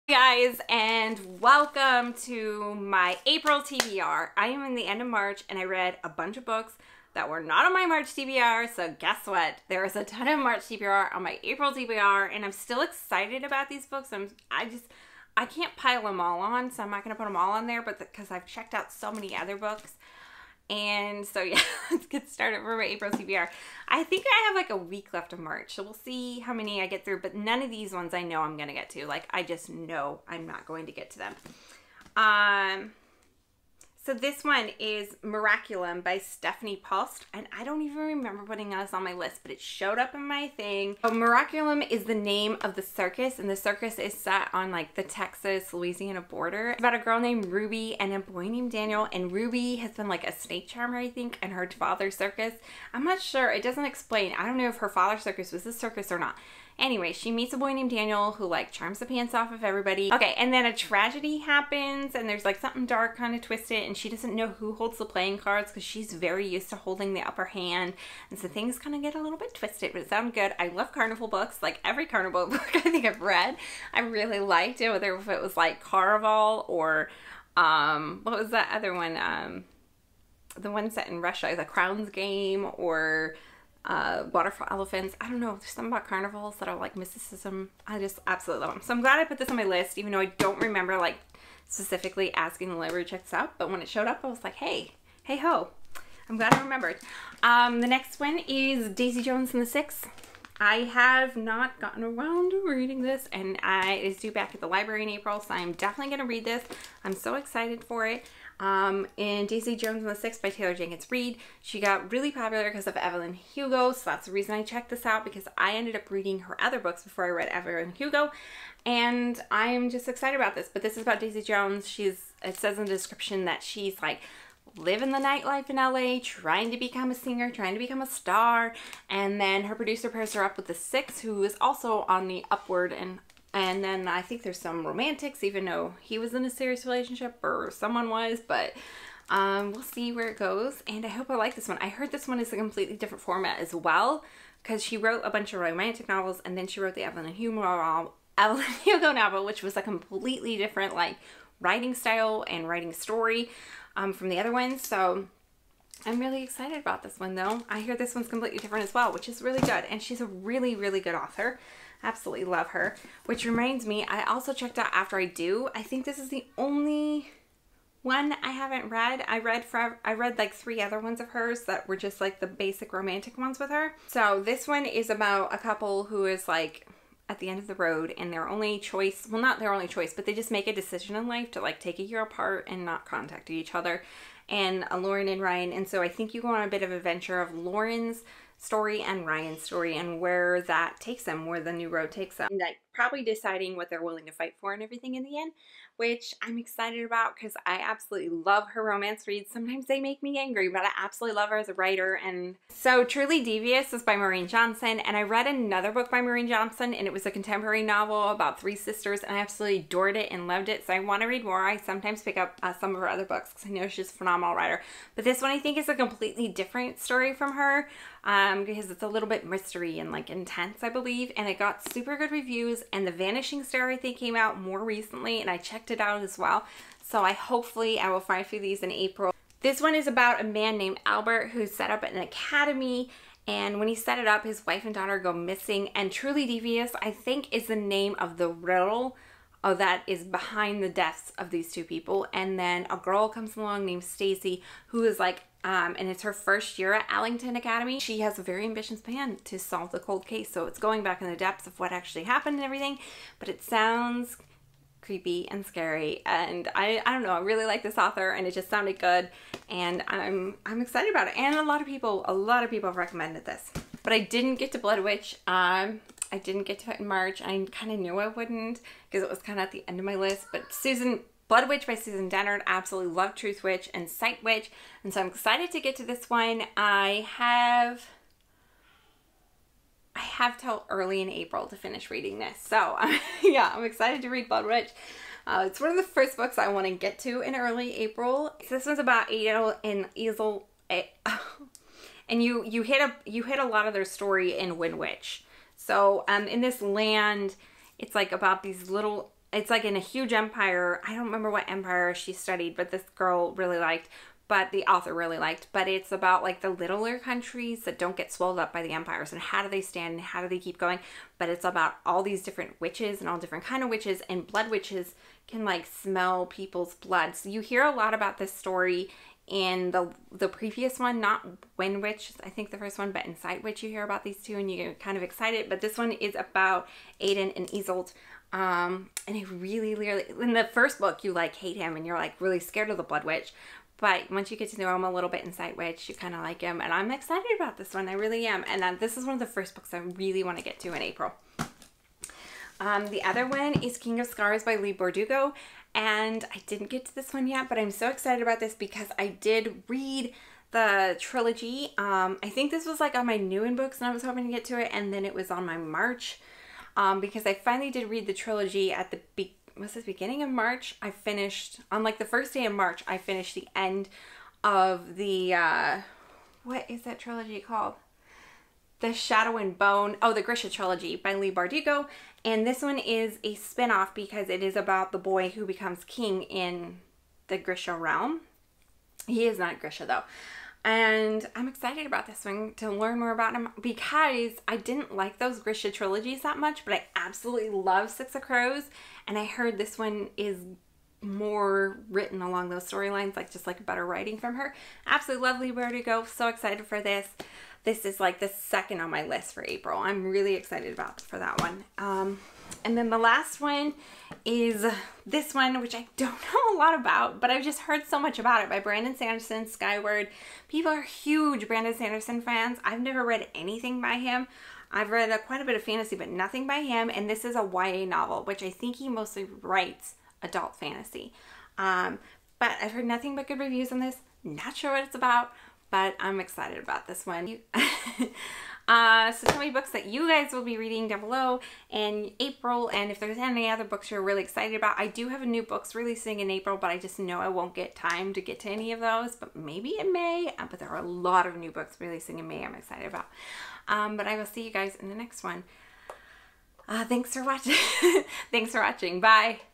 APRIL TBR | 2019
I have few books from my March TBR still left! I am so excited about all the reads I chose this month.